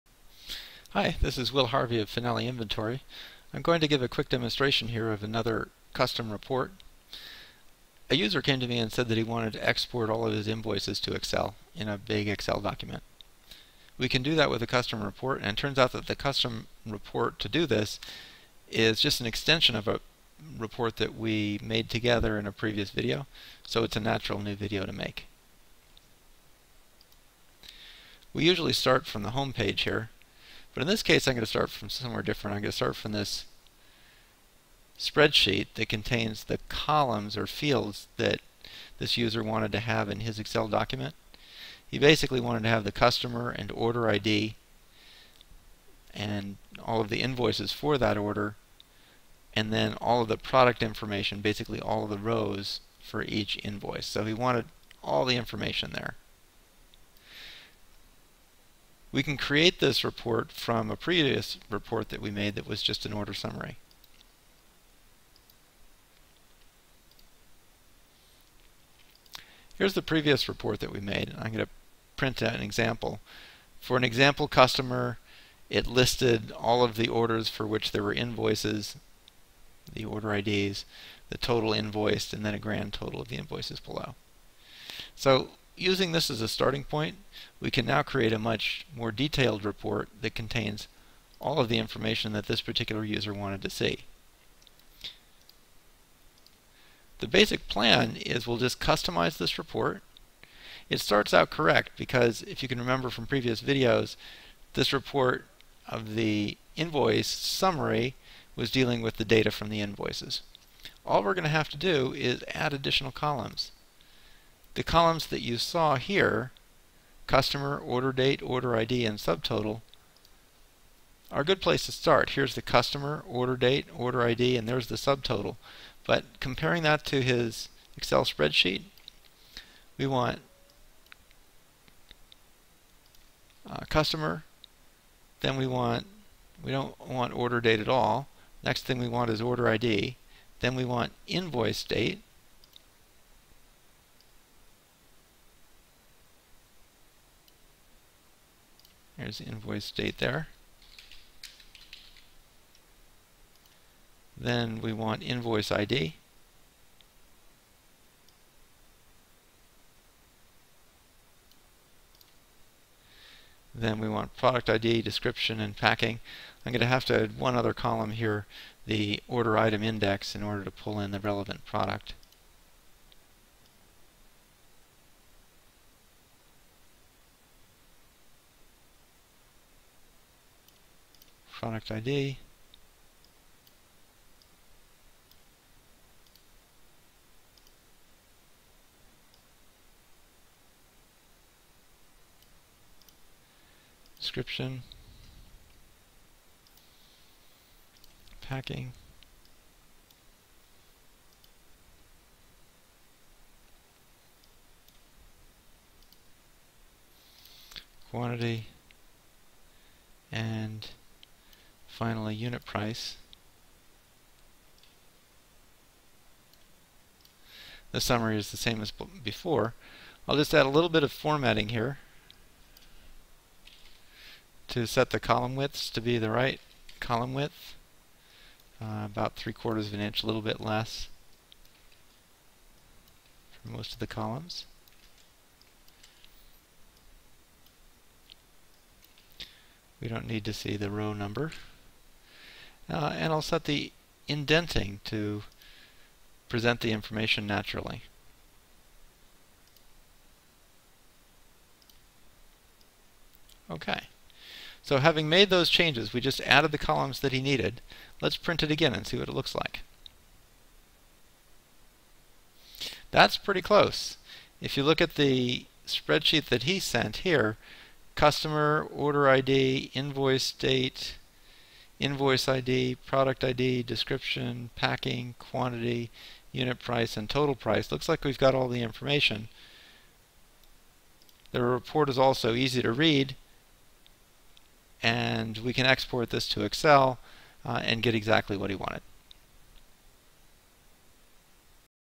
Creating custom report of invoices that you can export to Excel
Creating custom report of all invoice data that you can export to Excel